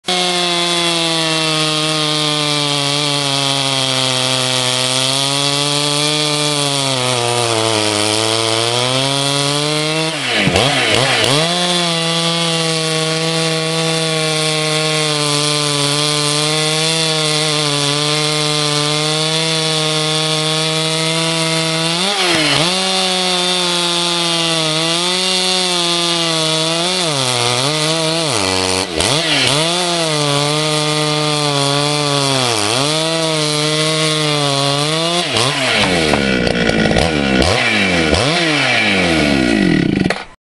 Husqvarna 395 Stock Part I
This is the biggest saw in the challenge. At least 2ccs more than the nearest competitor. Same bar and chain combo.